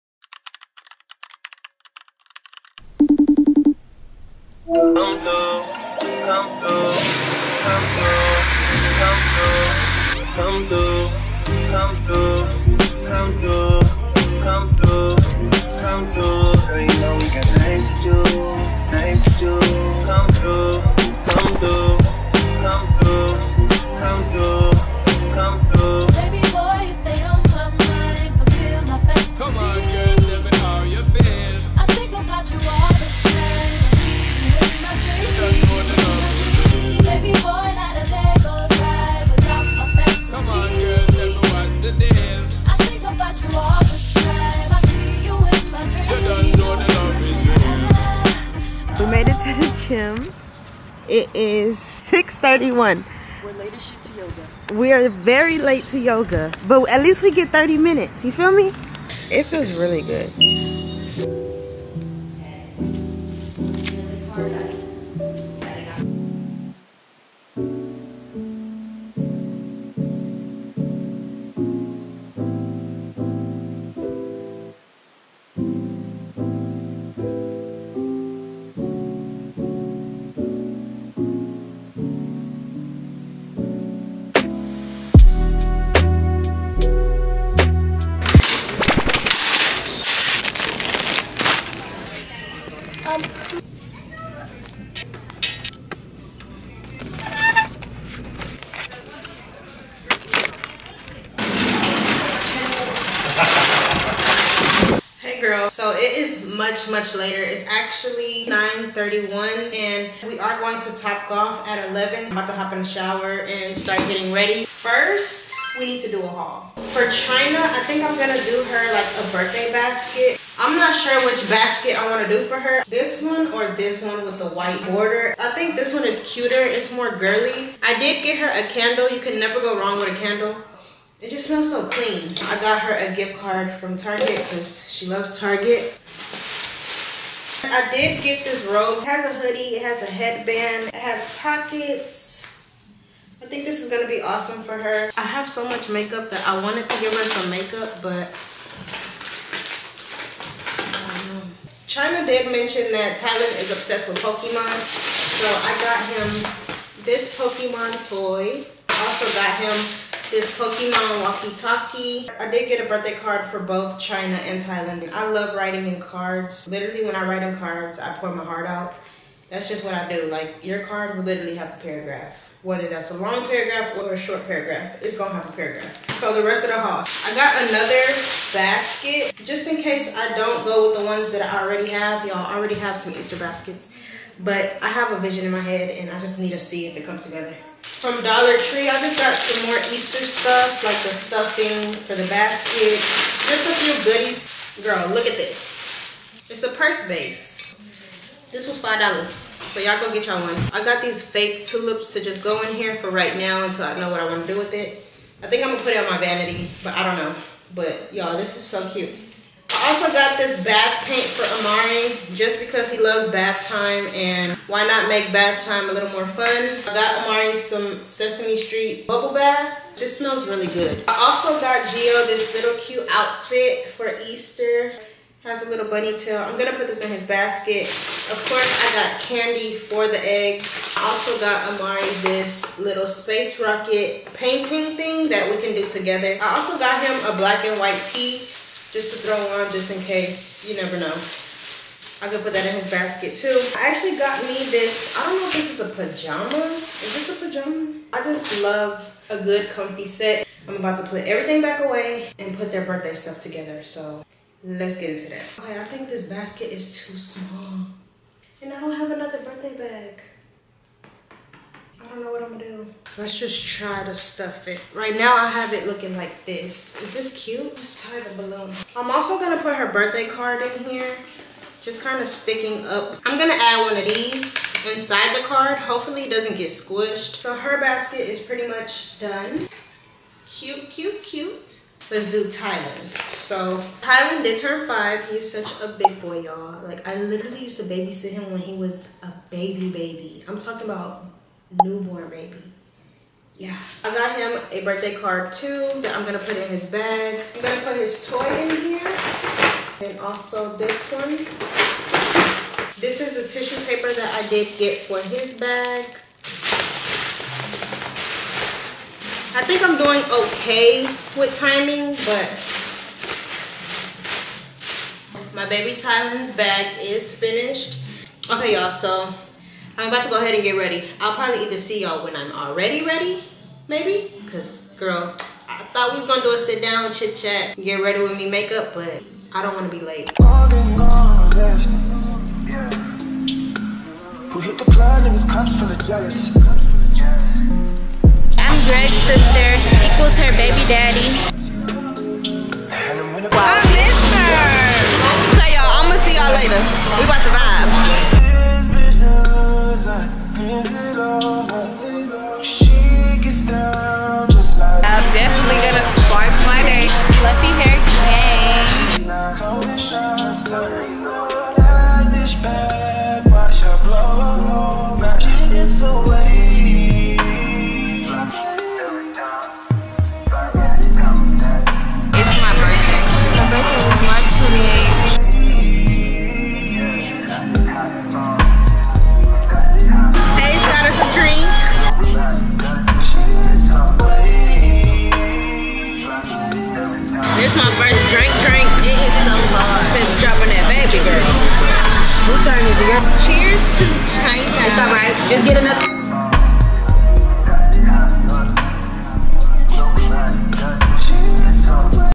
VLOG| a PRODUCTIVE day in my life✨ early morning vibes + target haul + top golf + yoga + Aries szn
Hey everyone! Today, I'm sharing a glimpse into my productive day. In today’s exciting video we will be spending a slow, relaxing morning together , attending a yoga class/gym, shopping, and celebrating my friend Chyna’s birthday which also is her precious son Tylan’s birthday too! HAPPY BIRTHDAY TO CHYNA AND TYAN!🥳 I love you both!

00:00 in today’s vlog
00:06 early morning vibes✨
00:49 yoga + gym
01:29 target run asmr
01:45 target haullll :)
04:15 assemble gifts: HAPPY BIRTHDAY Chyna & Tylan🥳❤️
05:30 topgolf